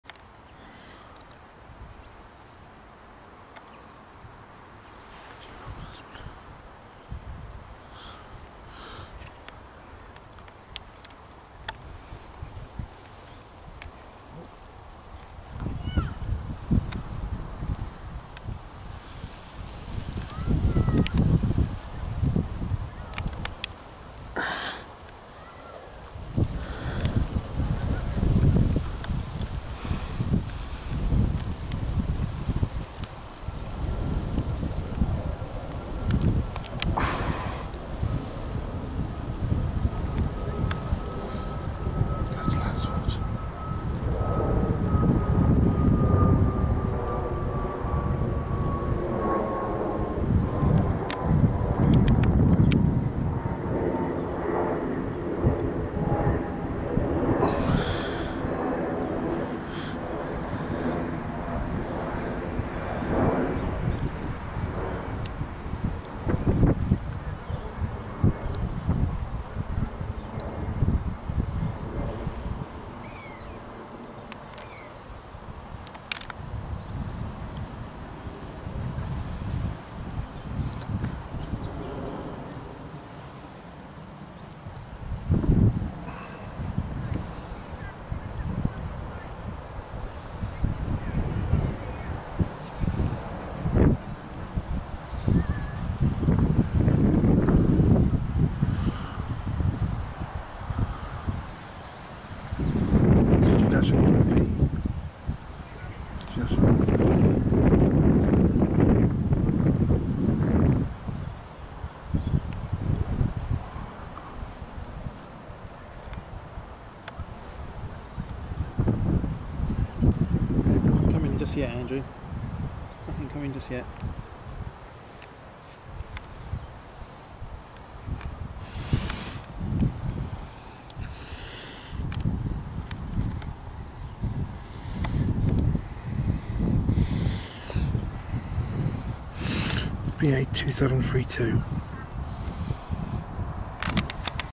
British Airways Airbus A321-251NX G-NEOV BA2732 climbing over Jealott's Hill to Lanzarote
British Airways Airbus A321-251NX G-NEOV flies over Jealott's Hill from Heathrow Airport to Lanzarote Airport on Saturday 30th October 2021.